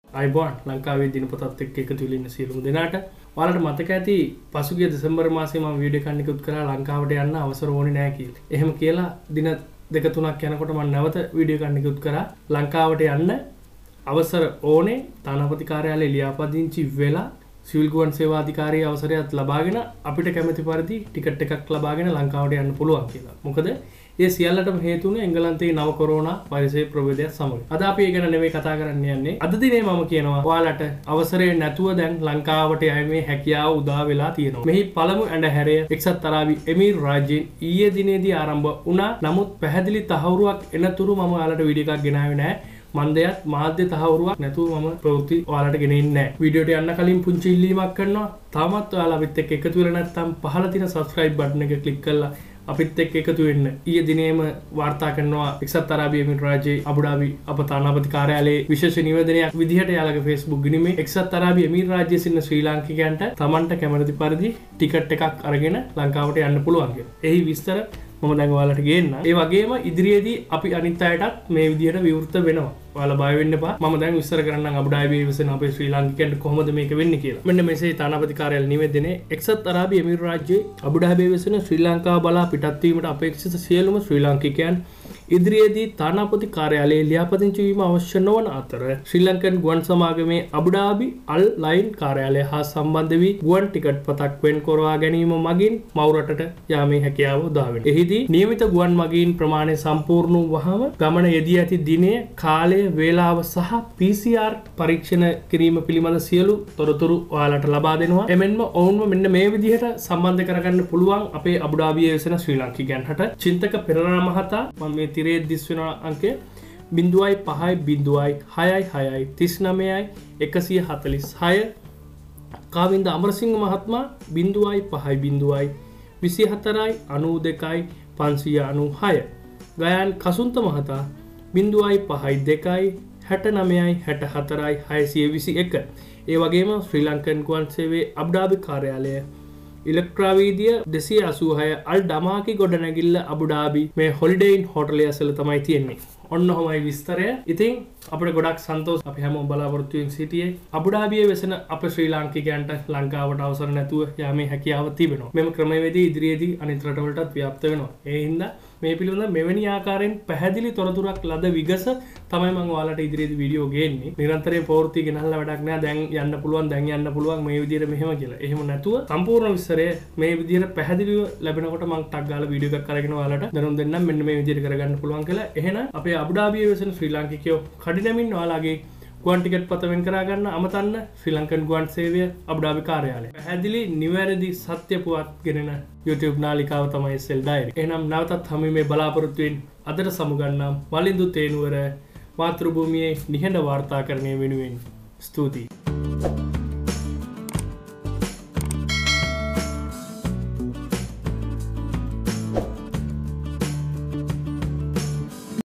Book your Ticket | fly to Sri Lanka | Start from today onwards
Book your Ticket | fly to Sri Lanka
එක්සත් අරාබි එමීර් රාජ්‍යයේ අබුඩාබි හි වෙසෙන ශ්‍රී ලංකාව බලා පිටත් වීමට අපේක්ෂිත සියලුම ශ්‍රී ලාංකිකයන් ඉදිරියේදී තානාපති කාර්යාලයේ ලියාපදිංචි වීම අවශ්‍ය නොවන අතර ශ්‍රී ලංකන් ගුවන් සමාගමේ අබුඩාබි/අල් අයින් කාර්යාලය හා සම්බන්ධ වී ගුවන් ටිකට්පතක් වෙන් කරවා ගැනීම මගින් මව්රට බලා පිටත් වීමට හැකියාව ඇත. නියමිත ගුවන්මගීන් ප්‍රමාණය සම්පූර්ණ වූ වහාම ගුවන් ගමන යෙදී ඇති දිනය, වේලාව, PCR පරීක්ෂණ පිළිබඳ විස්තර ආදී අවශ්‍ය සියලුම උපදෙස් සහ තොරතුරු එම ආයතනය මඟින් ලබා දෙනු ඇත.
පහත සඳහන් දුරකතන අංක ඔස්සේ ශ්‍රී ලංකන් ගුවන් සමාගමේ අබුඩාබි ශාඛාව හා සම්බන්ධ විය හැකිය.
ශ්‍රී ලංකන් ගුවන් සේවය – අබුඩාබි
ශ්‍රී ලංකන් ගුවන් සේවය – අල් අයින්
මුරාබා මාවත, අල් අයින් (ALAM සුපිරි වෙළඳසල අසල)